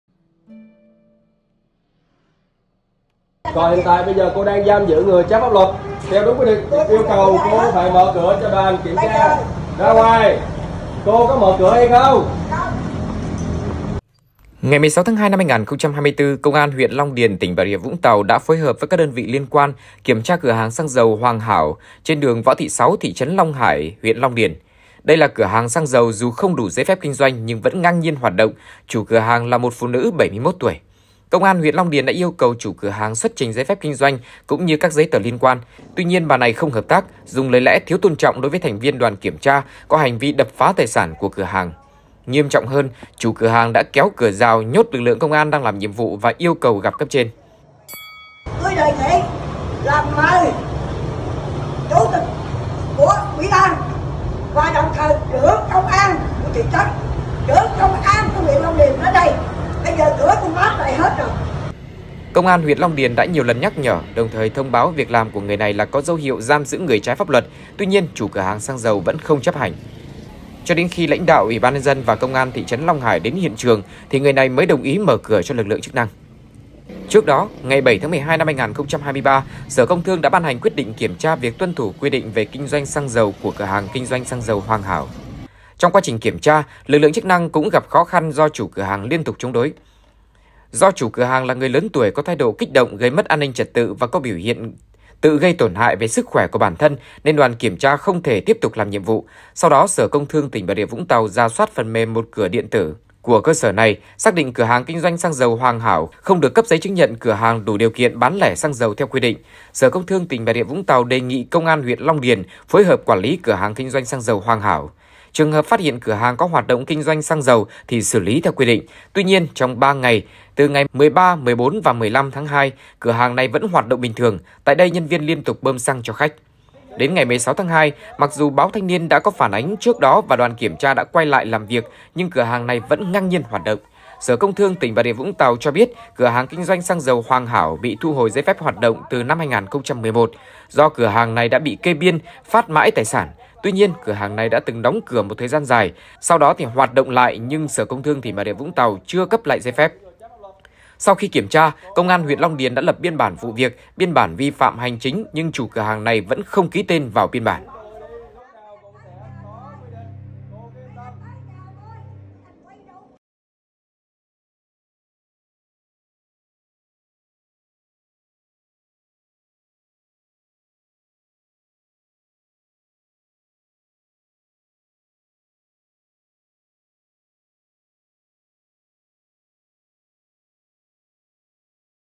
Chủ cửa hàng xăng dầu kéo cửa "nhốt" công an khi bị kiểm tra giấy phép
Mặc dù chưa được cấp giấy chứng nhận đủ điều kiện bán lẻ xăng dầu, nhưng cửa hàng kinh doanh xăng dầu Hoàng Hảo vẫn ngang nhiên hoạt động.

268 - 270 Nguyễn Đình Chiểu, Phường Võ Thị Sáu, Q.3, TP HCM